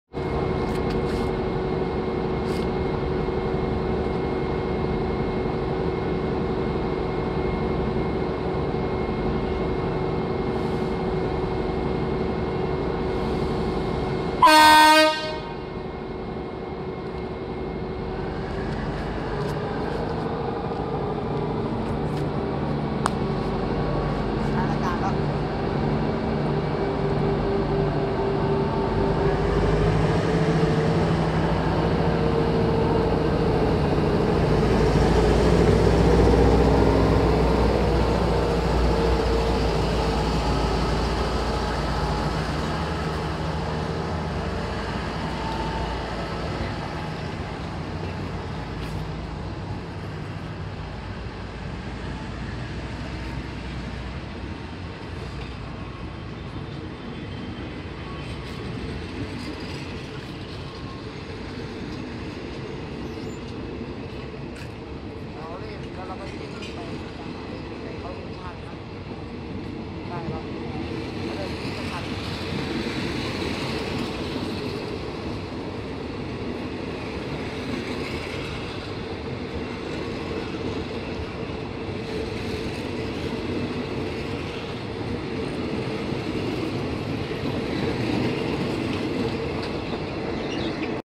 ขบวนรถเร็ว ขบวนที่ 142 ทำขบวนออกจากสถานีศรีสะเกษ Alsthom ALD. 4301 CAT.
ขบวนรถเร็วขบวนที่ 142 อุบลราชธานี - กรุงเทพ ทำขบวนออกจากสถานีศรีสะเกษ เมื่อวันที่ 12 มกราคม 2558 เวลา 17.57 น.